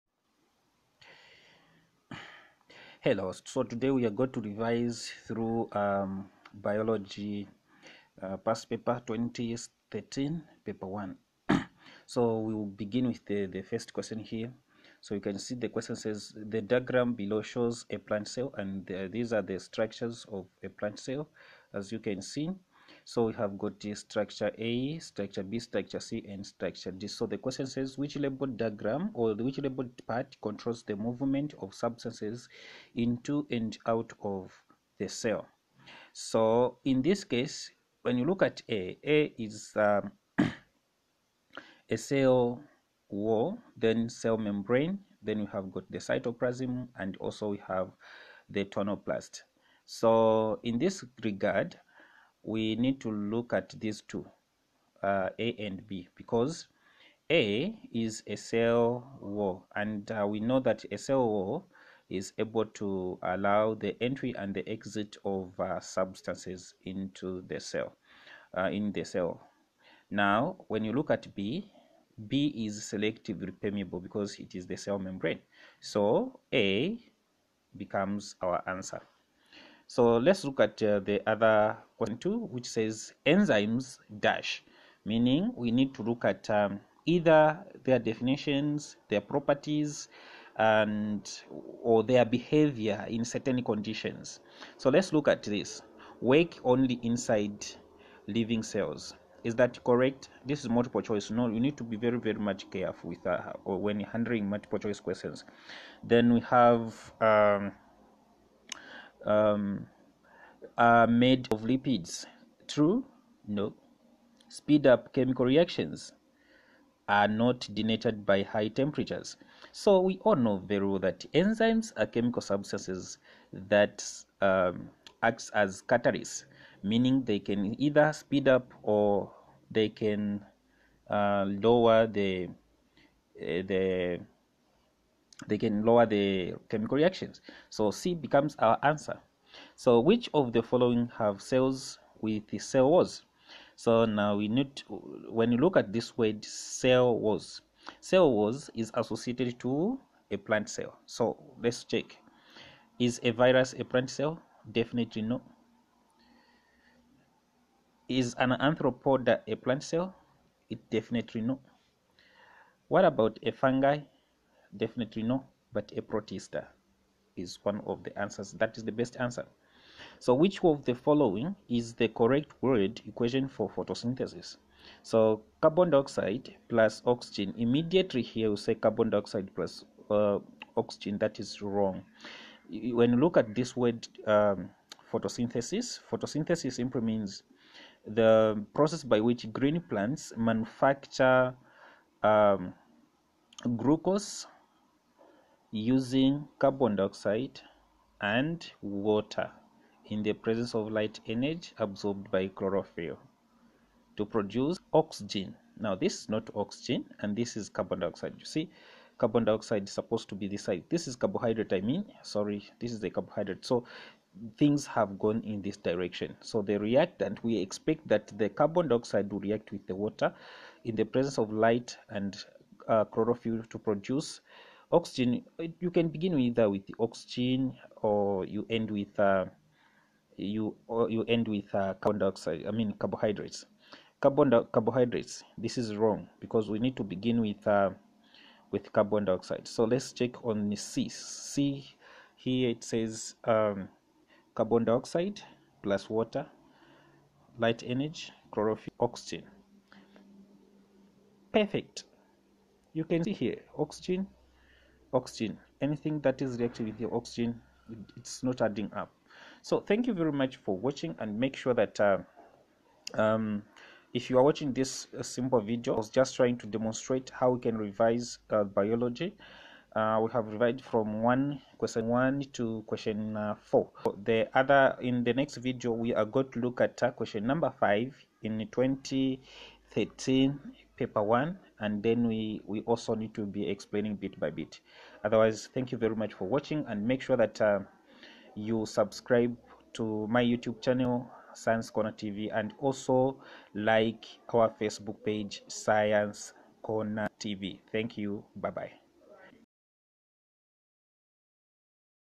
Biology Paper 1 Revision (2013)
This video highlights how to answer multiple choice questions in Biology paper 1.